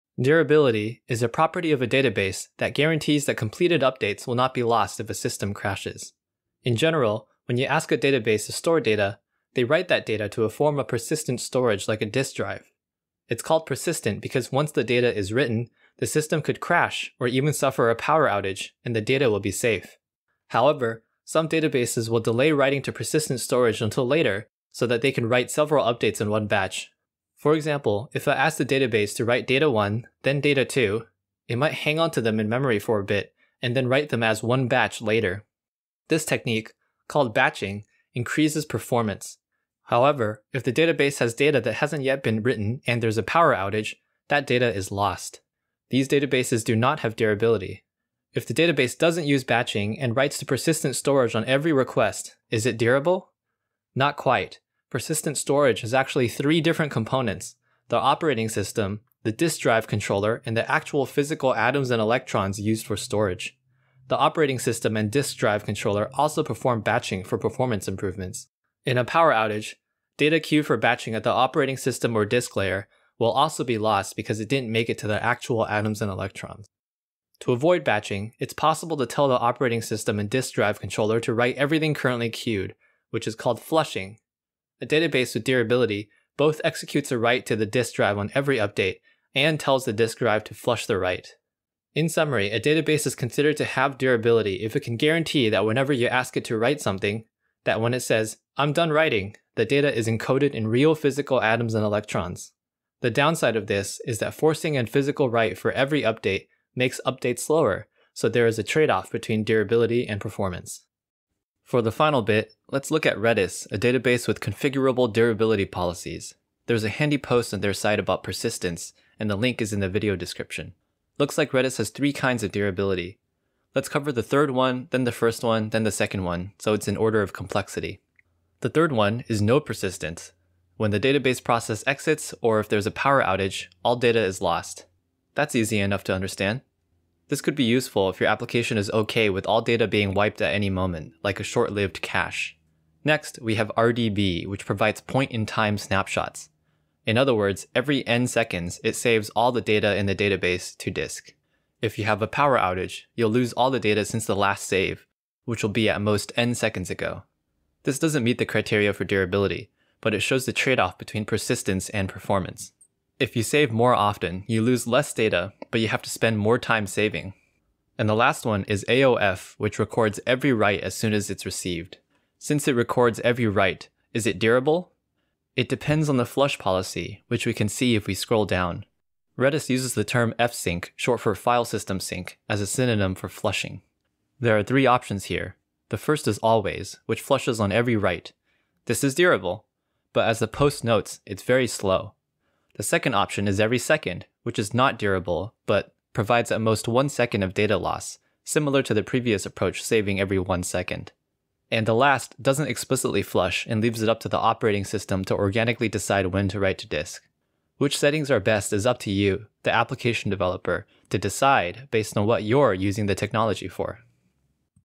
Durability, the D of ACID | Software Engineering Dictionary
Durability is a property of a database that guarantees that completed updates will not be lost if a system crashes.

In general, when you ask databases to store data, they write that data to a form of persistent storage like a disk drive. It’s called persistent because once the data is written, the system could crash, or even suffer a power outage, and the data will be safe.

However, some databases will delay writing to persistent storage until later so that they can write several updates in one batch. For example, if I ask the database to write data 1, then data 2, it might hang on to them in memory and then write them in one batch later. This technique, called batching, increases performance. However, if the database has data that hasn’t yet been written and there’s a power outage, that data is lost. These databases do _not_ have durability.

If the database doesn’t use batching and writes to persistent storage on every request, is it durable? Not quite. Persistent storage is actually three different components: the operating system, the disk drive controller, and the actual physical atoms and electrons used for storage. The operating system and disk drive controller also perform batching for performance improvements. In a power outage, data queued for batching at the operating system or disk layer will also be lost because it didn’t make it to the actual atoms and electrons.

To avoid batching, it’s possible to tell the operating system and disk drive controller to write everything currently queued, which is called flushing. A database with durability both executes a write to the disk drive on every update and tells the disk drive to flush the write.

In summary, a database is considered to have durability if it can guarantee that whenever you ask it to write something, that when it says “done writing”, the data is encoded in real physical atoms and electrons. The downside of this is that forcing a physical write for every update makes updates slower, so there is a tradeoff between durability and performance.

For the final bit, let’s look at Redis, a database with configurable durability policies. There’s a handy post on their site about persistence, and the link is in the video description. Looks like Redis has three kinds of durability. Let’s cover the third one, then the first one, then the second one, so it’s in order of complexity. The third one is no persistence. When the database process exits or there’s a power outage, all data is lost. That’s easy enough to understand. It could be useful if your application is OK with all data being wiped at any moment, like a short-lived cache. Next, we have RDB, which provides point-in-time snapshots. In other words, every N seconds, it saves all the data in the database to disk. If you have a power outage, you’ll lose all the data since the last save, which will be at most N seconds ago. This doesn’t meet the criteria for durability, but it shows the tradeoff between persistence and performance. If you save more often, you lose less data, but you have to spend more time saving. And the last one is AOF, which records every write as soon as it’s received. Since it records every write, is it durable? It depends on the flush policy, which we can see if we scroll down. Redis uses the term fsync, short for filesystem sync, as a synonym for flushing. There are three options here. The first is always, which flushes on every write. This is durable! But as the post notes, it’s very slow. The second option is every second, which is not durable, but provides at most 1 second of data loss, similar to the previous approach saving every 1 second. And the last doesn’t explicitly flush and leaves it up to the operating system to organically decide when to write to disk. Which settings are best is up to you, the application developer, to decide, based on what you’re using the technology for.

00:00 Intro
00:07 What Is Persistent Storage?
00:21 Uh Oh: Batching Makes Databases Not Durable
00:49 The Operating System and Disk Drive Also Batch
01:17 Tell Them Not To Batch With The Flush Command
01:32 Summary
01:52 Redis Durability Research